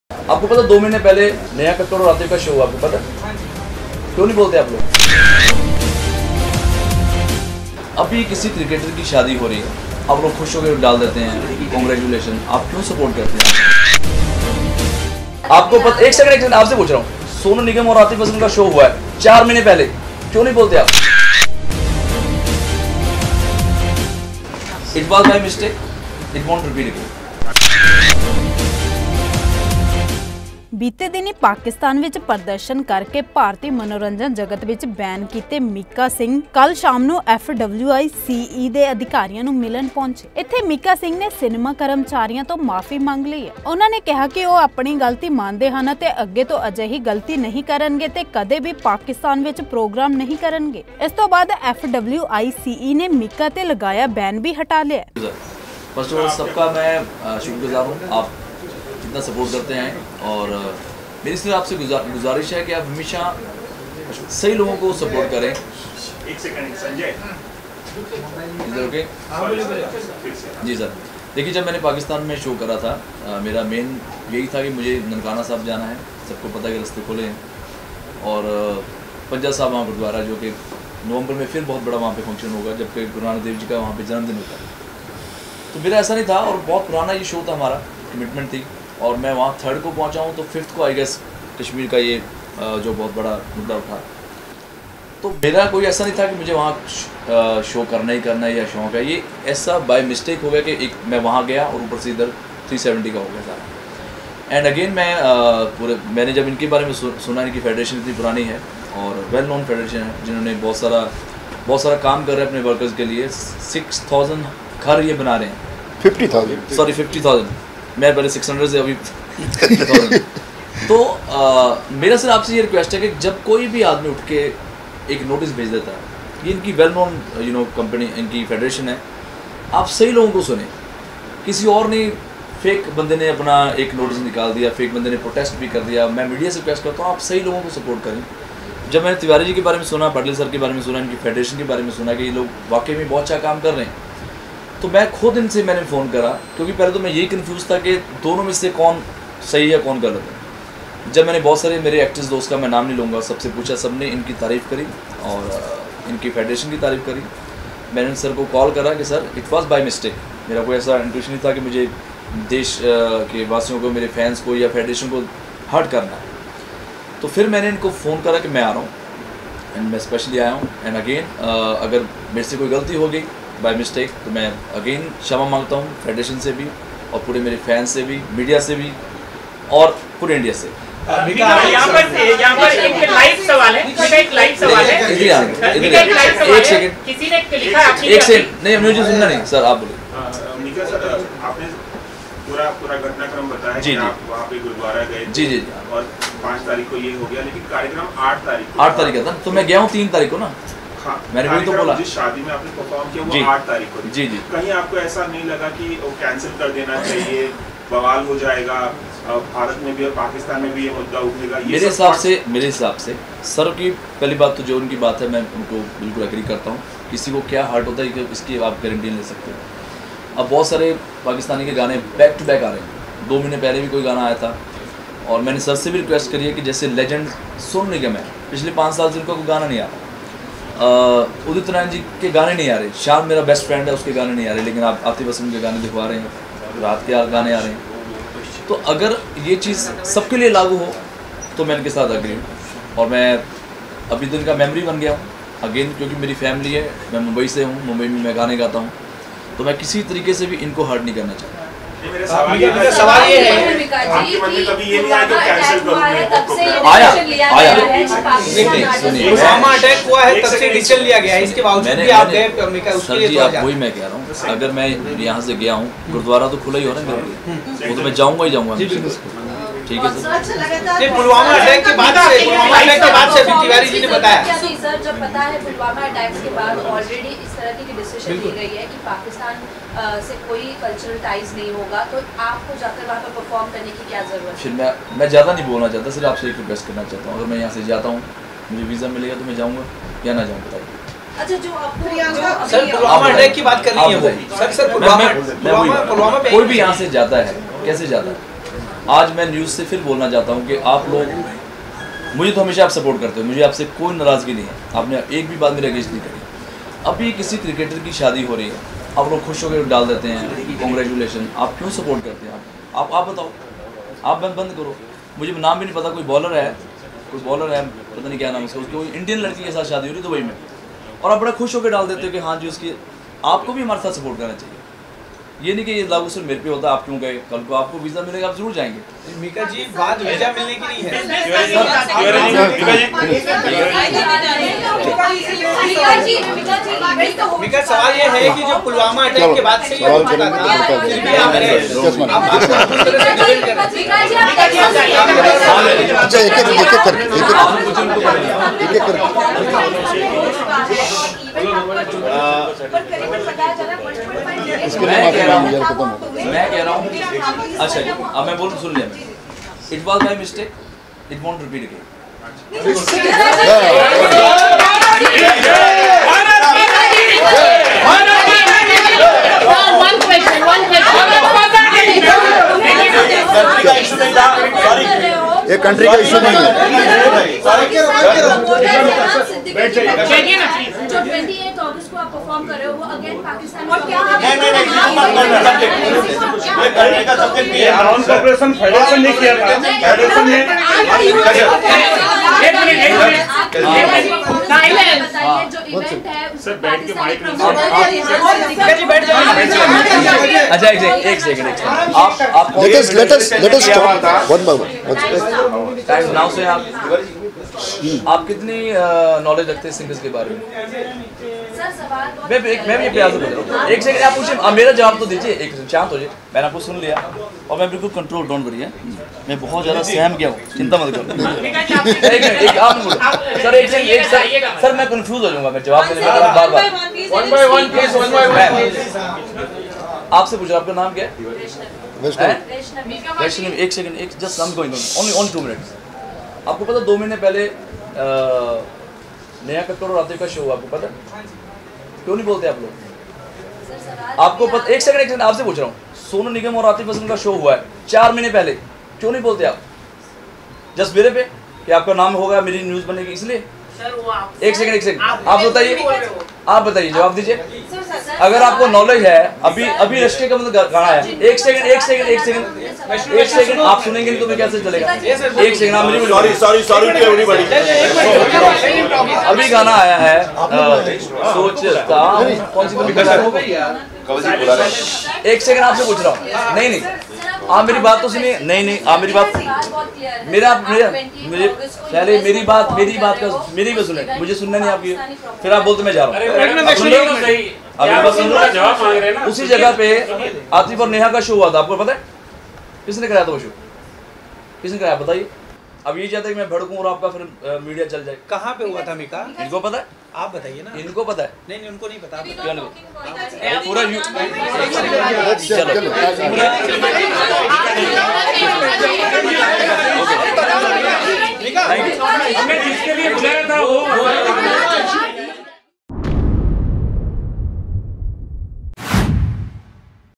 Mika Singh | Mika Singh's Ban Lifted | Why am I being singled out?
Mika Singh questioned other Bollywood singers who performed in Pakistan in last months.
Sonu Nigam, Neha Kaka

"If the government of India is giving Visa, then nothing wrong in that" Mika singh